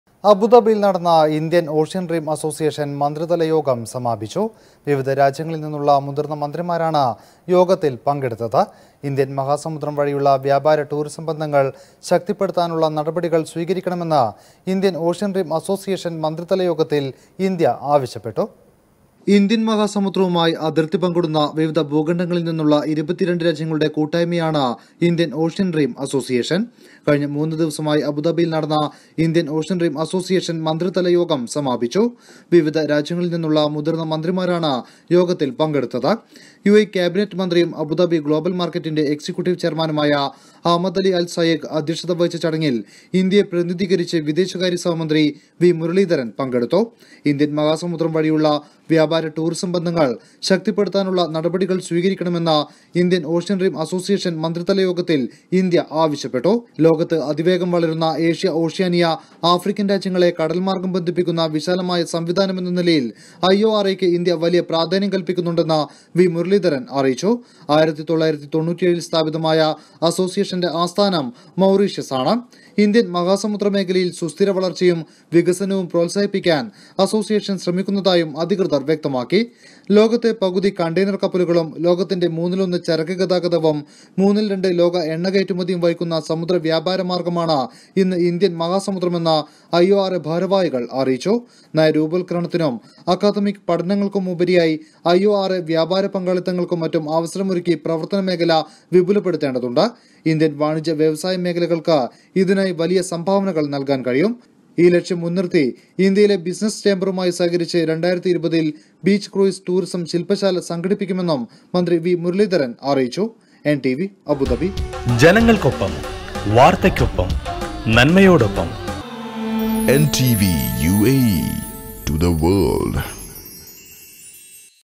അബുദബിയില്‍ നടന്ന ഇന്ത്യന്‍ ഓഷ്യന്‍ റീം അസോസിയേഷന്‍ മന്ത്രിതല യോഗം സമാപിച്ചു.
വിവിധ രാജ്യങ്ങളില്‍ നിന്നുള്ള മുതിര്‍ന്ന മന്ത്രിമാരാണ് യോഗത്തില്‍ പങ്കെടുത്തത്.
ഇന്ത്യന്‍ മഹാസമുദ്രം വഴിയുള്ള വ്യാപാര ടൂറിസം ബന്ധങ്ങള്‍ ശക്തിപ്പെടുത്താനുള്ള നടപടികള്‍ സ്വീകരിക്കണമെന്ന് ഇന്ത്യന്‍ ഓഷ്യന്‍ റീം അസോസിയേഷന്‍ മന്ത്രിതലയോഗത്തില്‍ ഇന്ത്യ ആവശ്യപ്പെട്ടു.